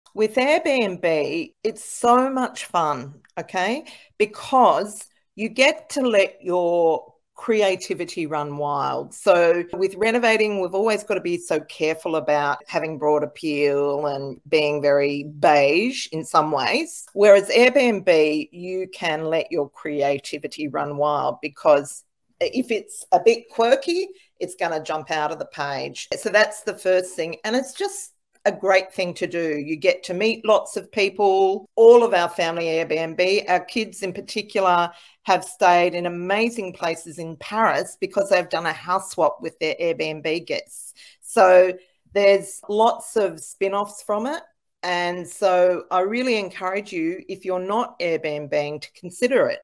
Get ready to unleash your creativity with Airbnb!
Get ready to unleash your creativity with Airbnb! Join Bernadette Janson on Episode 189 as she shares how this platform offers fun, quirky and exciting opportunities for hosting. Don't miss out on amazing experiences and meet new people! Tune in now!